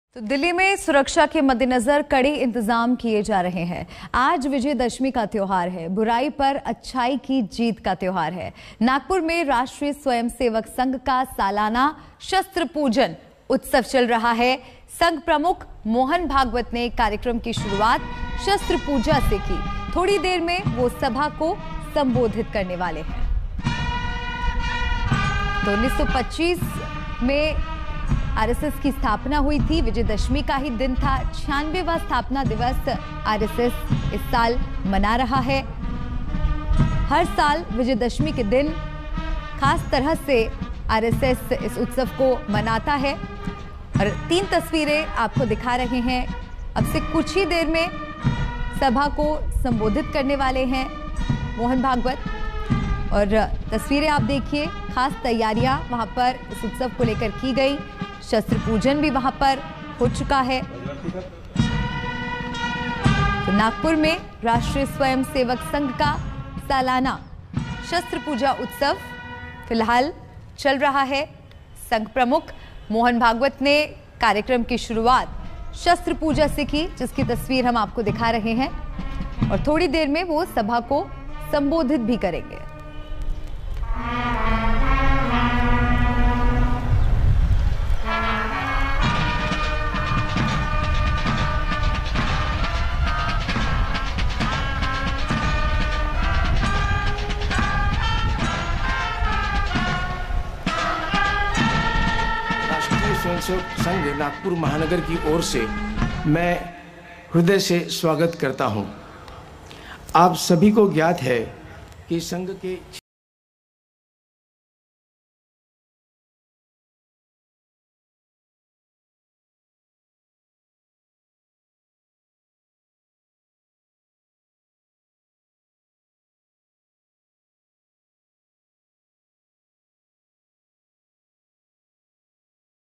Vijayadashmi पर संघ प्रमुख Mohan Bhagwat ने की शस्त्र पूजा, थोड़ी देर में होगा संबोधन I Latest News
RSS चीफ Mohan Bhagwat  ने Vijayadashmi  के मौके पर 'शस्त्र पूजा' की. यह कार्यक्रम Maharashtra के Nagpur में हुआ. Vijayadashmi के दिन ही साल 1925 में RSS की स्थापना हुई थी. थोड़ी देर में संघ प्रमुख Mohan Bahagwat का संबोधन होगा.